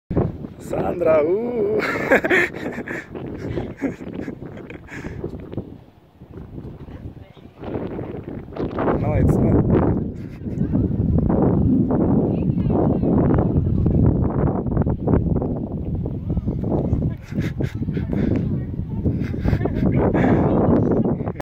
Dangerous but beautiful view | Boville | Boville tirane 2019
we were hiking in the reservoir of bovilla, Tirana. the view was amazing, but it was dangerous too.